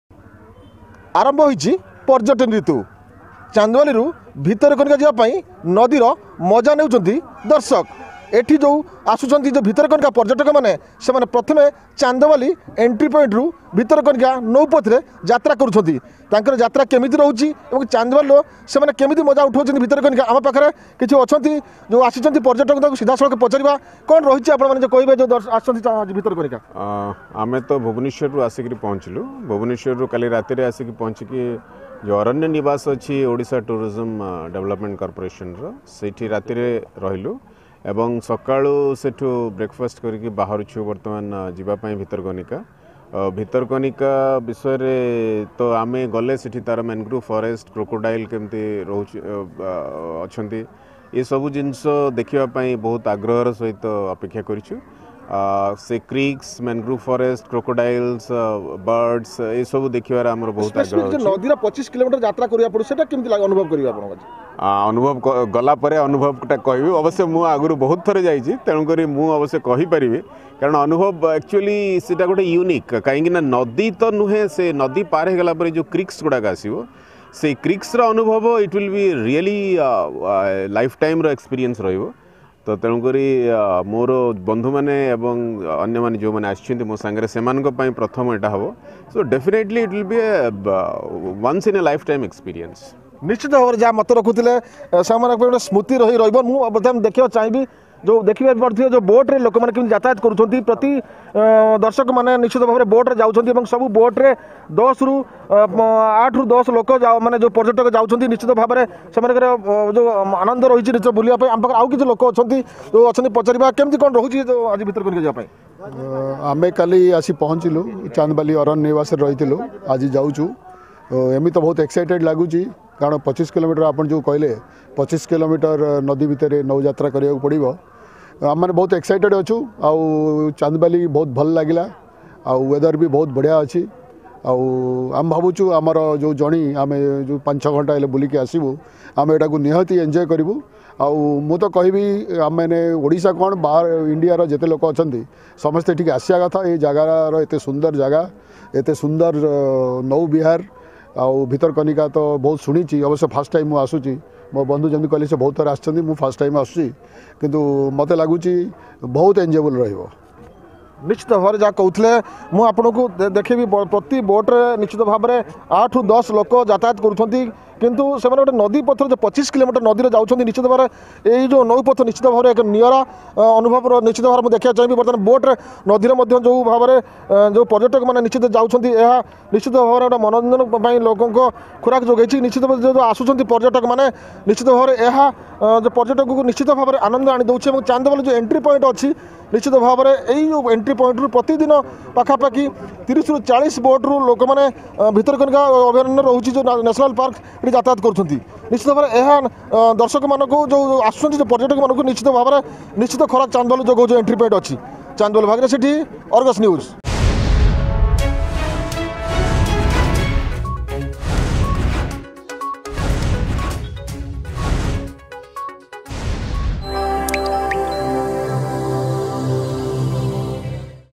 ଆରମ୍ଭ ହୋଇଛି ପର୍ଯ୍ୟଟନ ଋୁତୁ
ଶୁଣନ୍ତୁ ଭିତରକନିକାକୁ ଆସୁଥିବା ପର୍ଯ୍ୟଟକମାନଙ୍କ ପ୍ରତିକ୍ରିୟା .
ଅଧିକ ଖବର ପାଇଁ ଡ଼ାଉନଲୋଡ କରନ୍ତୁ “Argus News” ଆପ୍